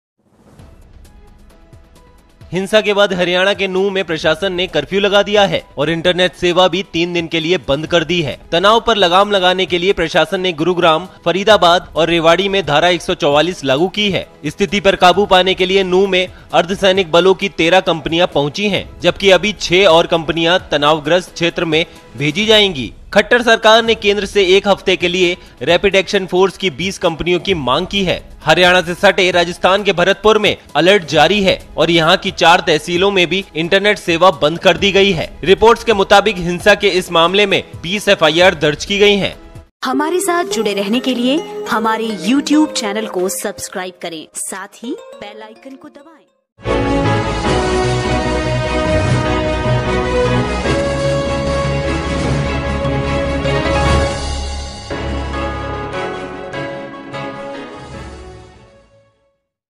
हरियाणा : हिंसा के बाद नूंह में लगाया गया कर्फ्यू, अर्धसैनिक बलों की 13 टुकड़ियां पहुंची|Gaya24x7
हरियाणा : हिंसा के बाद नूंह में लगाया गया कर्फ्यू, अर्धसैनिक बलों की 13 टुकड़ियां पहुंची।